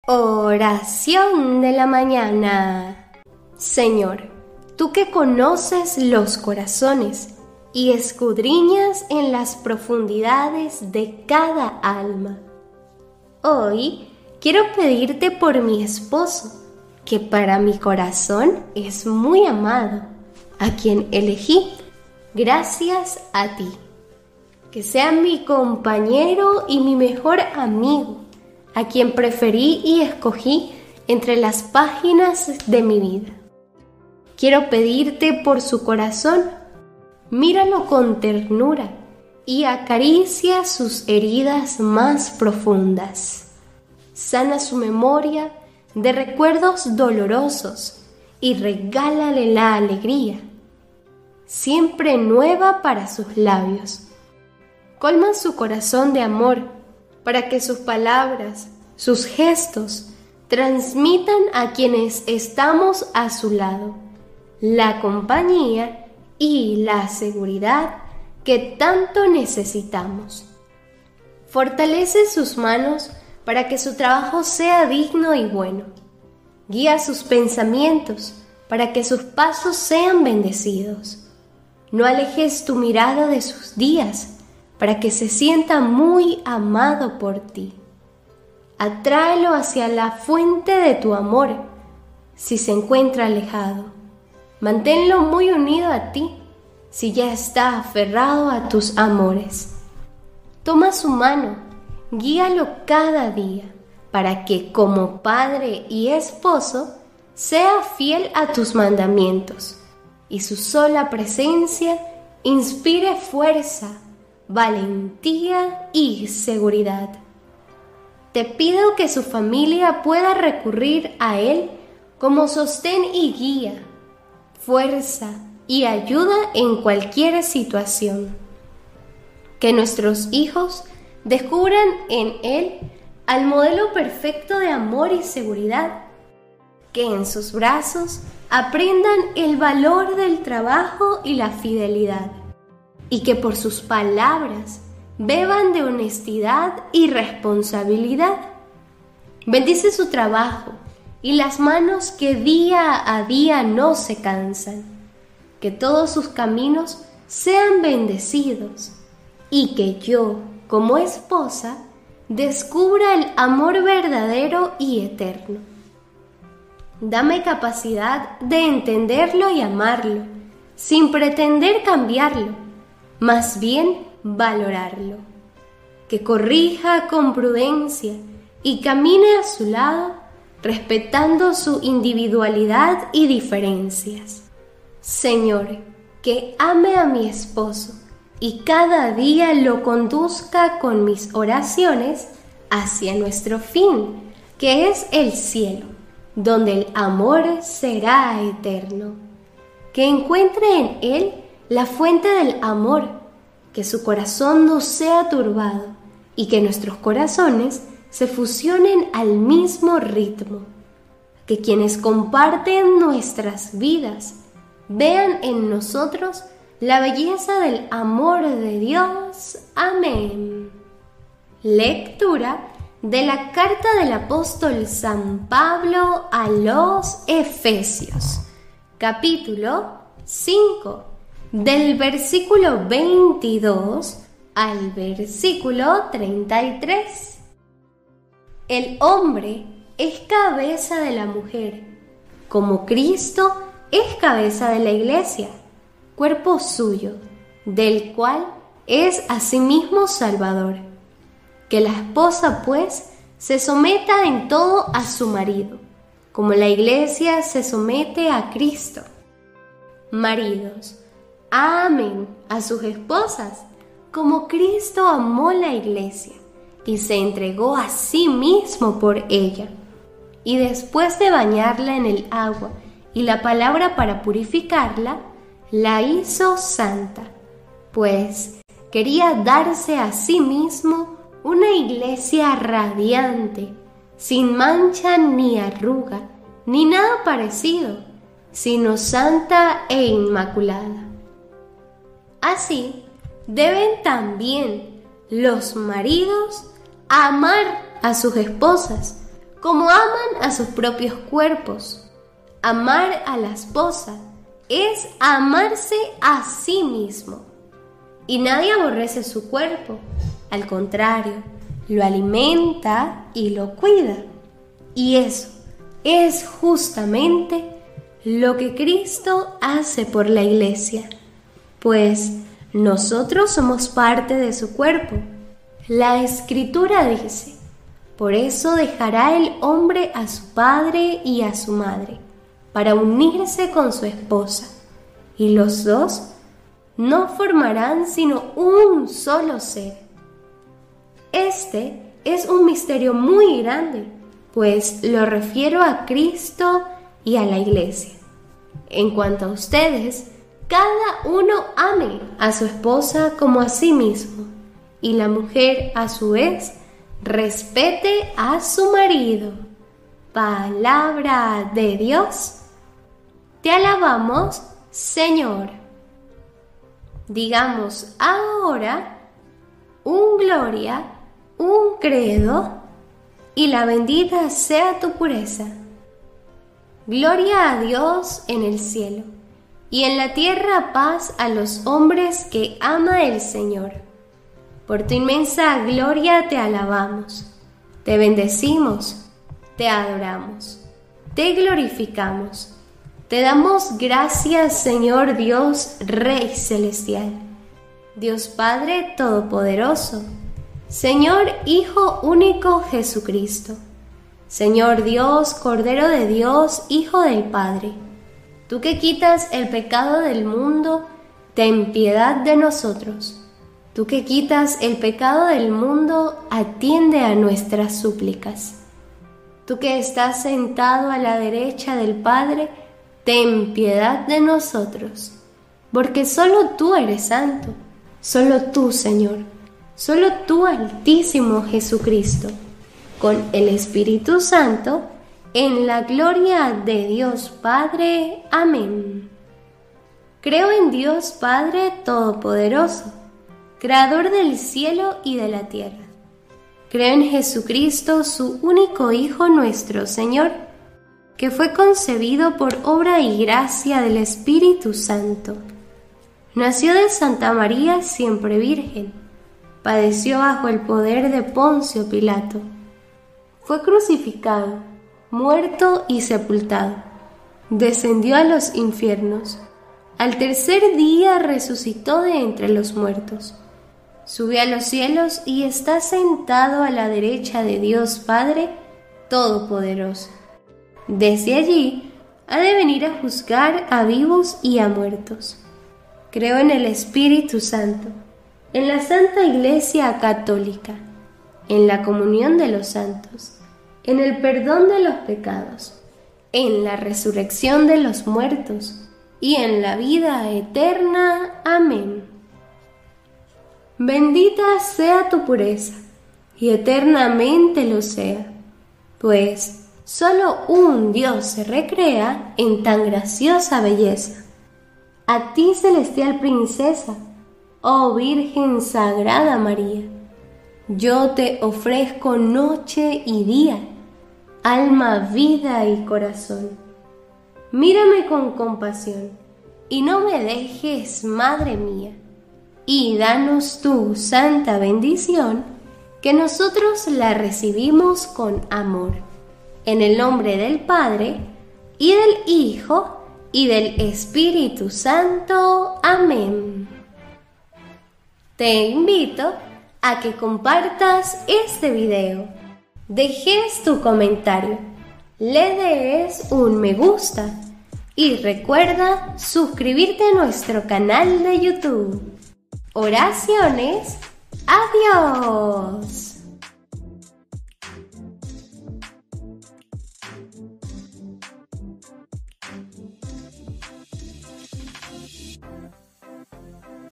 ORACION de la MAÑANA de HOY ☀️ Domingo 27 de Febrero 2022 🙏 ORACIONES A DIOS
¿quieres apoyar al padre marcos?

A ti te imploro Señor, desde la mañana oyes mi voz
Sean bienvenidos queridos hermanos al canal de Oraciones a Dios
Hagamos esta oración con fe y devoción

Señor, Tú que conoces los corazones y escudriñas en las profundidades de cada alma, hoy quiero pedirte por mi esposo, que para mi corazón es muy amado, a quien elegí gracias a Ti. Que sea mi compañero y mi mejor amigo, a quien preferí y escogí entre las páginas de mi vida.

Quiero pedirte por su corazón, míralo con ternura y acaricia sus heridas más profundas. Sana su memoria de recuerdos dolorosos y regálale la alegría siempre nueva para sus labios. Colma su corazón de amor, para que sus palabras, sus gestos transmitan a quienes estamos a su lado la compañía y la seguridad que tanto necesitamos.

Fortalece sus manos para que su trabajo sea digno y bueno, guía sus pensamientos para que sus pasos sean bendecidos. No alejes tu mirada de sus días, para que se sienta muy amado por Ti. Atráelo hacia la Fuente de Tu Amor, si se encuentra alejado; manténlo muy unido a Ti, si ya está aferrado a tus amores.

Toma su mano y guíalo cada día para que como padre y esposo, sea fiel a tus mandamientos y su sola presencia inspire fuerza, valentía y seguridad.
Te pido que su familia pueda recurrir a él, como sostén y guía, fuerza y ayuda en cualquier situación.

Que nuestros hijos descubran en él, al modelo perfecto de amor y seguridad. Que en sus brazos aprendan del valor del trabajo y la fidelidad, y que por sus palabras beban de honestidad y responsabilidad.

Bendice su trabajo y las manos que día a día no se cansan. Que todos sus caminos sean bendecidos y que yo, como esposa, descubra el amor verdadero y eterno.

Dame capacidad de entenderlo y amarlo, sin pretender cambiarlo, más bien valorarlo. Que corrija con prudencia y camine a su lado respetando su individualidad y diferencias.

Señor, que ame a mi esposo y cada día lo conduzca con mis oraciones hacia nuestro fin, que es el Cielo, donde el amor será eterno. Que encuentre en él la fuente del amor, que su corazón no sea turbado y que nuestros corazones se fusionen al mismo ritmo. Que quienes comparten nuestras vidas, vean en nosotros la belleza del Amor de DIOS en nuestras vidas. Amén.

Lectura de la carta del Apóstol San Pablo a los Efesios 5,22-33
El hombre es cabeza de la mujer, como Cristo es cabeza de la Iglesia, cuerpo suyo, del cual es asimismo salvador. 24.Que la esposa, pues, se someta en todo a su marido, como la Iglesia se somete a Cristo. 25.Maridos, amen a sus esposas como Cristo amó a la Iglesia y se entregó a sí mismo por ella. 26.Y después de bañarla en el agua y la Palabra para purificarla, la hizo santa, 27.pues quería darse a sí mismo una Iglesia radiante, sin mancha ni arruga ni nada parecido, sino santa e inmaculada. 28.Así deben también los maridos amar a sus esposas como aman a sus propios cuerpos: amar a la esposa, es amarse a sí mismo. 29.Y nadie aborrece su cuerpo; al contrario, lo alimenta y lo cuida. Y eso es justamente lo que Cristo hace por la Iglesia, 30.pues nosotros somos parte de su cuerpo. 31.La Escritura dice: Por eso dejará el hombre a su padre y a su madre para unirse con su esposa, y los dos no formarán sino un solo ser. 32.Es éste un misterio muy grande, pues lo refiero a Cristo y a la Iglesia. 33.En cuanto a ustedes, cada uno ame a su esposa como a sí mismo, y la mujer, a su vez, respete a su marido."

Palabra de Dios, te alabamos Señor

Digamos ahora
Gloria
Credo
Bendita Sea Tu Pureza

Te invito a que compartas este video, dejes tu comentario, le des un me gusta